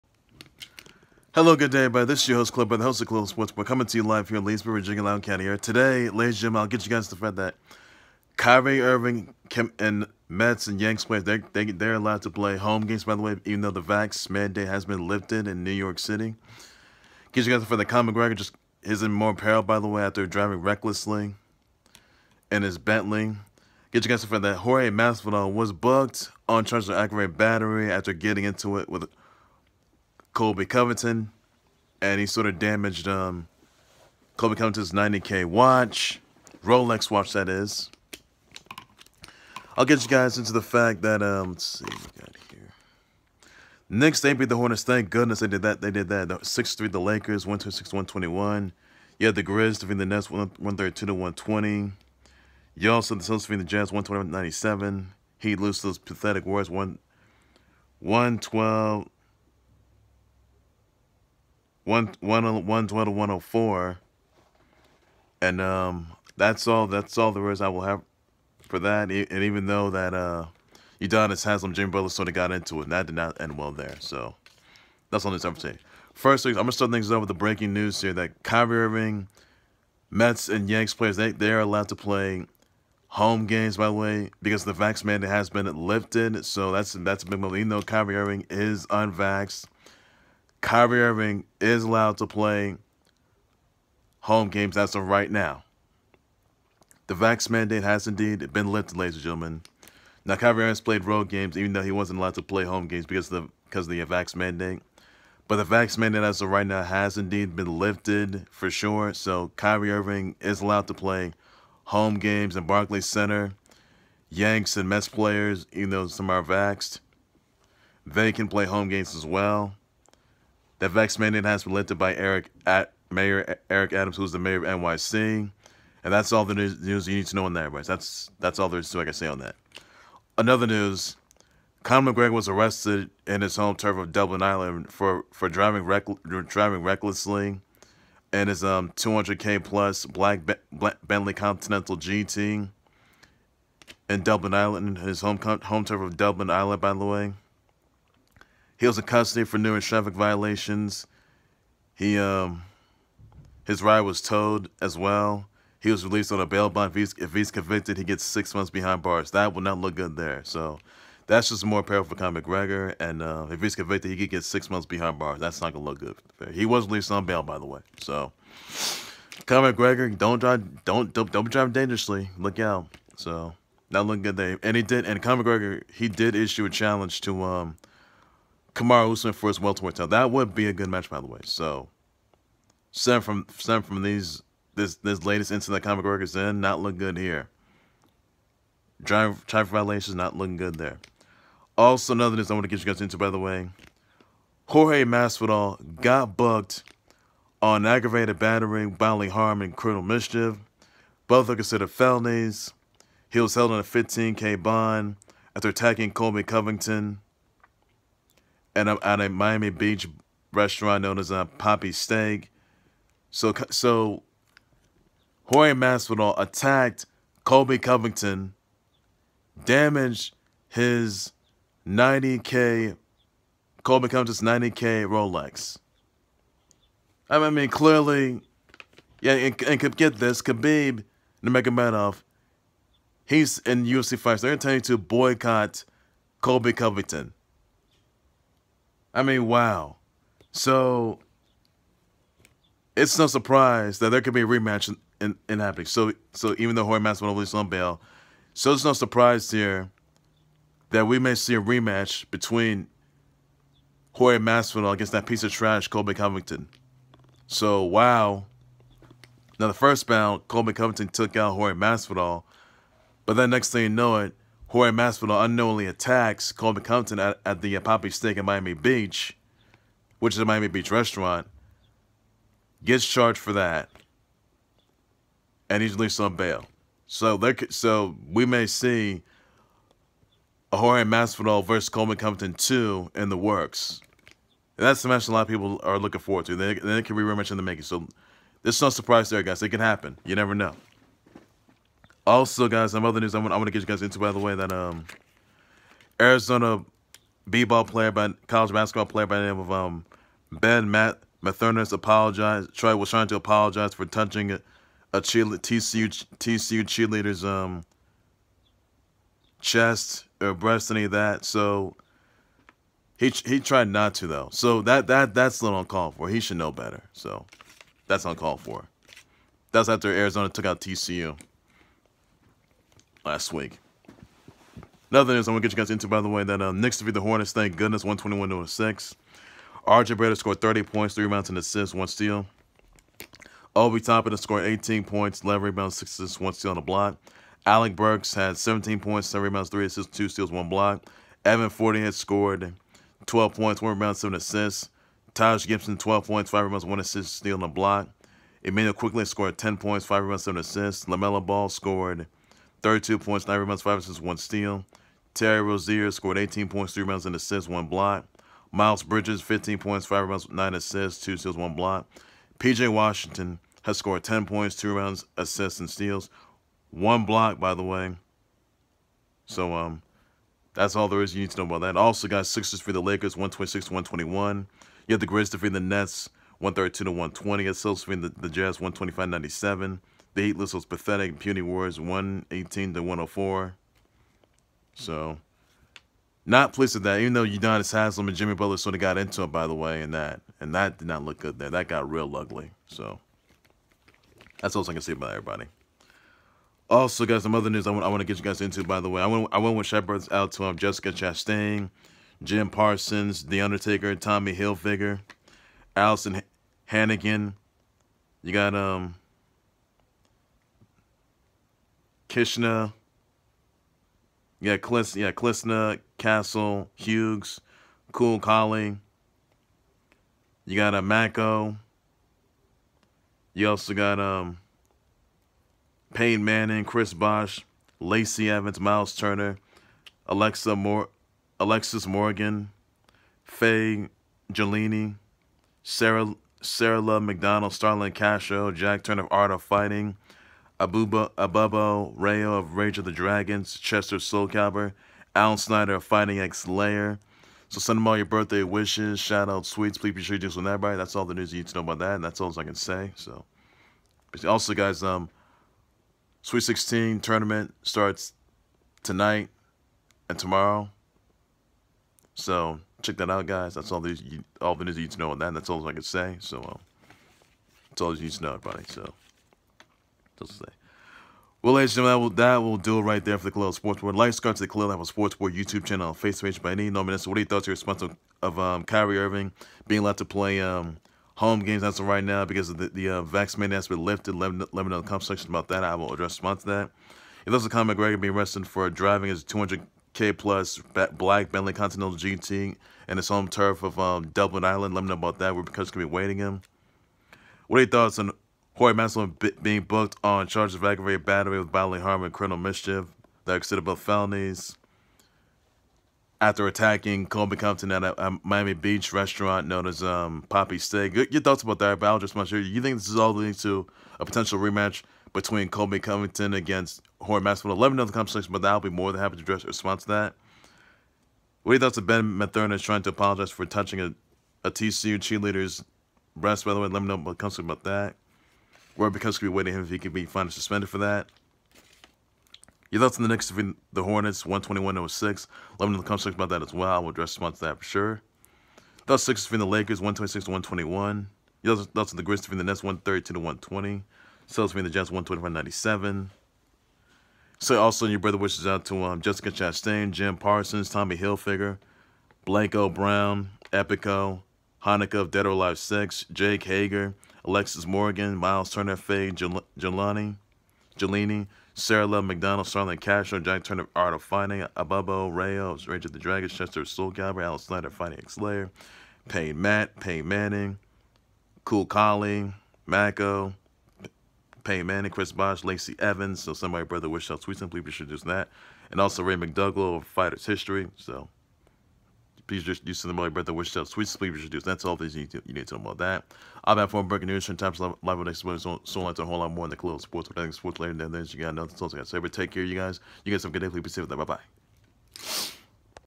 🚨BREAKING: NYC vax mandate lifted allowing Kyrie Irving to play home games, Conor McGregor arrested
🚨BREAKING: NYC vax mandate lifted allowing Kyrie Irving, Mets & Yanks players to play games at home.

Conor McGregor arrested for driving his $200K+ Black Bentley Continental GT ride dangerously in his home country of Dublin, Ireland. McGregor was in custody for numerous traffic violations, ride got towed, released on bail.

Jorge Masvidal booked on charges for aggravated battery with bodily harm & criminal mischief, both felonies, held on $15K bond after attacking Colby Covington in Miami Beach at Papi’s steak, Masvidal bailed out. Covington’s $90K Rolex watch damaged.

Knicks def Hornets 121-106:
RJ Barrett: 30 Pts, 3 Reb & AST, 1 STL
Obi Toppin: 18 Pts, 11 Reb, 6 AST, 1 STL & BLK
Alec Burks: 17 Pts, 7 Reb, 3 AST, 2 STL, 1 BLK
Evan Fournier: 12 Pts, 1 Reb, 7 AST
Taj Gibson: 12 Pts, 5 Reb, 1 AST, STL & BLK
Immanuel Quickley: 10 Pts, 5 Reb, 7 AST
Lamelo Ball: 32 Pts, 9 Reb, 5 AST, 1 STL
Terry Rozier: 18 Pts, 3 Reb & AST, 1 BLK
Miles Bridges: 15 Pts, 5 Reb, 9 AST, 2 STL, 1 BLK
PJ Washington: 10 Pts, 2 Reb, AST & STL, 1 BLK

Sixers def Lakers 126-121

Grizz def Nets 132-120

Celtics def Jazz 125-97

Heat lose to puny Warriors 112-104, leading to Jimmy Buckets & Udonis Haslem getting into it.

Benn Mathonis trying to apologize for touching TCU cheerleader’s chest.

Sweet 16 Tonight & Tomorrow

Happy B-Days: Jessica Chastain, Jim Parsons, Tommy Hilfiger, The Undertaker, Alyson Hannigan, Klisna Castle-Hughes, Kool Kollie, Macco, Peyton Manning, Chris Bosh, Lacey Evans, Blanco Brown, Epico, Ray McDougal of Fighter’s History, Honoka of DOA6, Jake Hager, Alexas Morgan, Myles Turner, Faye Gulini, Sarah Love MacDonald, Starlin Castro, Jack Turner of AOF, Chester of SC, Abubo Rao of Rage of The Dragons, Allen Snider of Fighting Ex Layer 🧁🍰🎂🎁🥳🎉🎊🎈